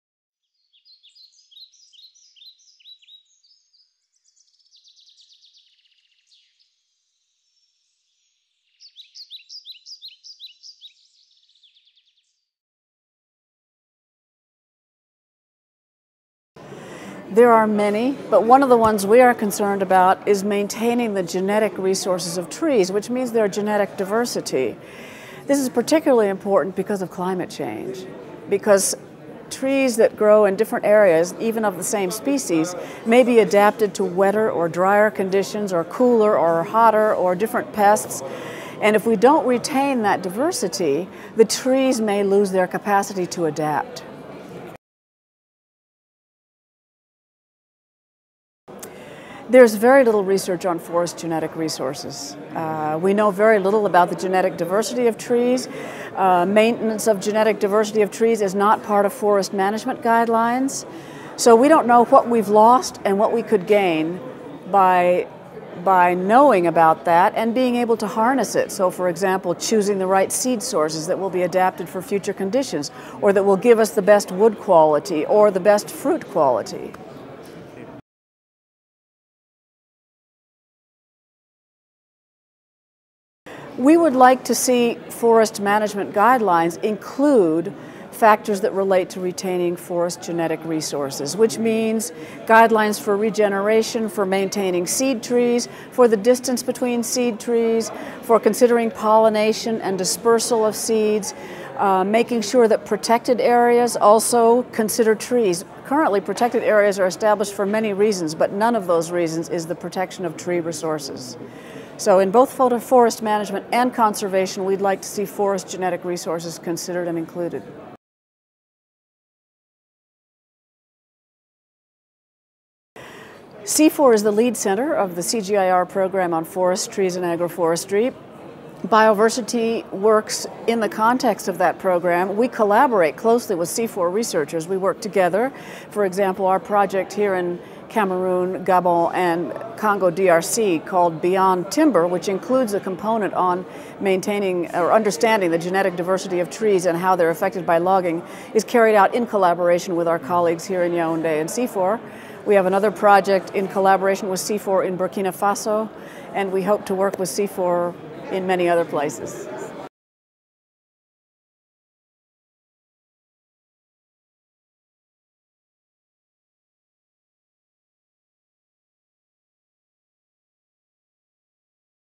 Q+A: Maintaining tree genetic diversity
Laura Snook Program Leader, Forest Genetic Resources at Bioversity speaks about the state of research on forest genetic resources.